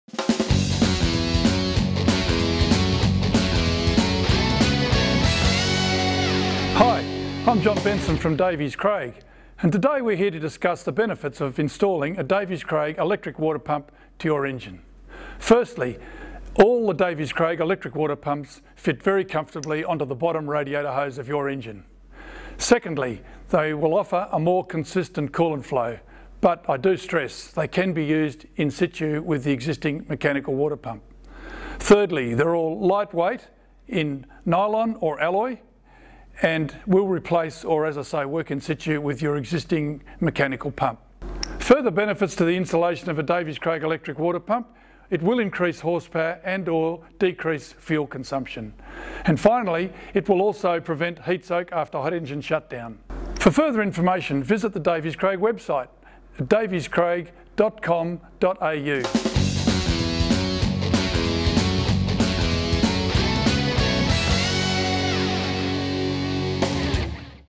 FAQ EWP Part 1 - What Are The Benefits Of An Davies, Craig Electric Water Pump (EWP)
In our series of FAQ's answered, this is Part 1 of 7
We explain in simple terms the benefits of a Davies, Craig EWP.

FAQ #1 Davies Craig’s remote-mounted Electric Water Pumps (EWP®) can be installed on the bottom radiator hose and can replace or will operate in situ with the existing mechanical water pump.
  Old mechanical, belt-driven water pumps run directly off the engine and sap engine power.
  As a performance or booster pump accessory that increases power sent to the drive wheels
  The engine cooling is improved with an EWP® by having a higher flow rate at idle and low engine speeds when there is little or no ram air, and when the engine is switched off.
  Improves cooling capacity and fuel economy offering a more consistent coolant flow
  Will eliminate damaging heat soak after a hot-engine shut down
  The significant improvement - the power the mechanical water pump absorbs from the engine increases as the cube of its speed – therefore when the pump speed doubles from idle, say 600 rpm to 1200 rpm, the power it absorbs increases 8 times, then another 8 times going to 2400 rpm and so on up to 10,000 rpm. This extra power and torque you are releasing by disabling the mechanical coolant pump, is delivered to the drive train.
  Electric Water Pumps can be hard wired into your vehicle’s electrical system or you may opt to use the LCD EWP/Fan Digital Controller which forms part of the EWP® Combo Kits.
  The LCD EWP®/Fan Digital Controller (Part # 8002, if ordered separately) used with either model EWP® will greatly enhance management of coolant flow, commensurate with the rising and falling of engine temperature as well as temperature controlling the operation of the electric Thermatic Fan/s.